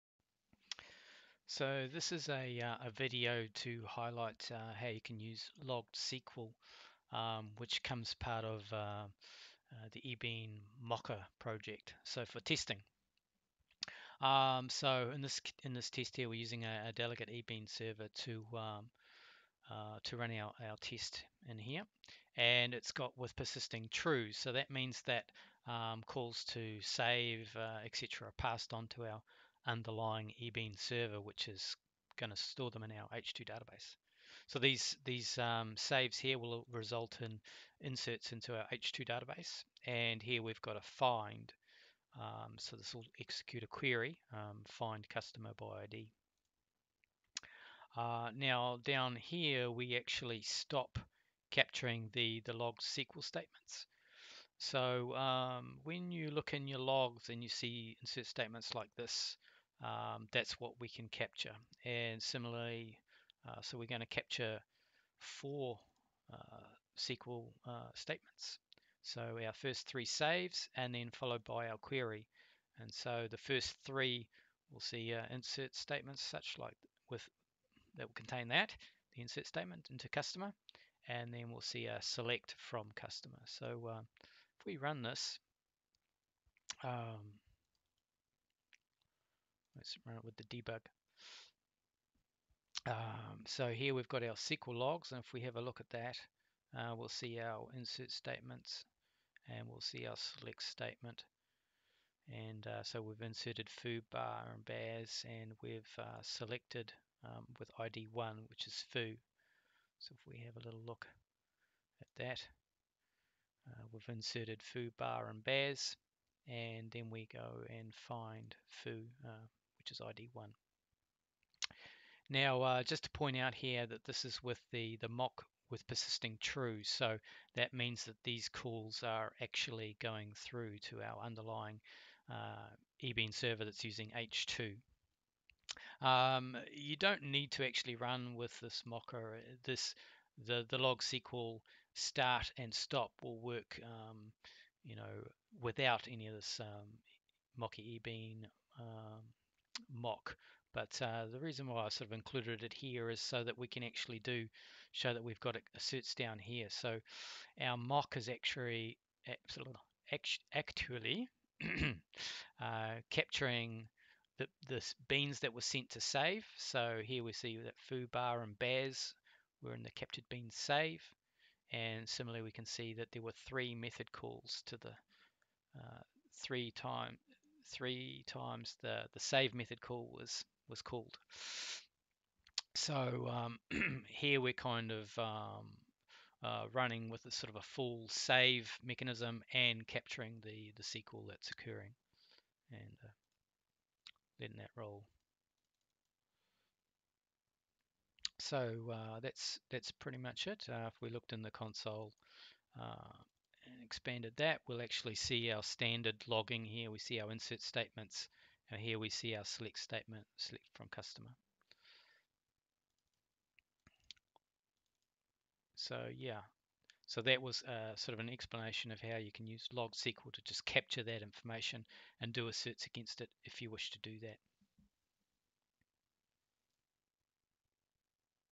Ebean ORM Testing - Using LoggedSql to capture sql statements
LoggedSql is part of the avaje-ebeanorm-mocker project.  It can be used in testing to catch sql that is executed and assert